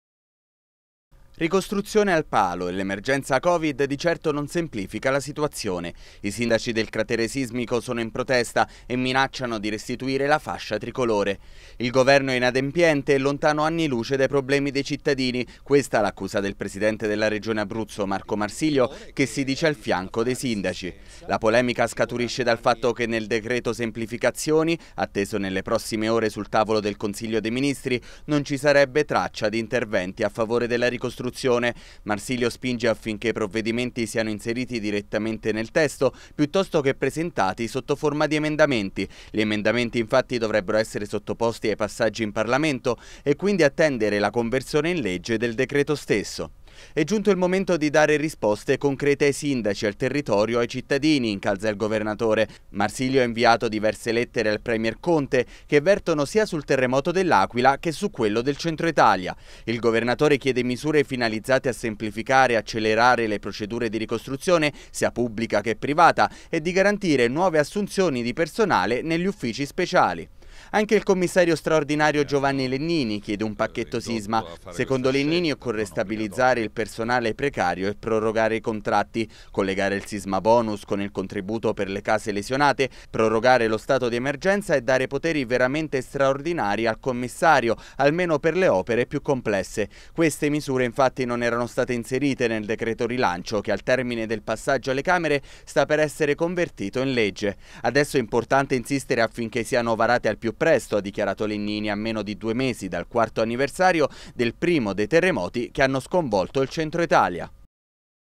Sisma. Marsilio: "Nel dl Semplificazioni nulla per la ricostruzione"
Il governatore Marco Marsilio si dice al fianco dei sindaci del cratere sismico per chiedere al premier Conte di inserire nel dl Semplificazioni misure per la ricostruzione. Anche il Commissario Straordinario Giovanni Legnini chiede un pacchetto sisma.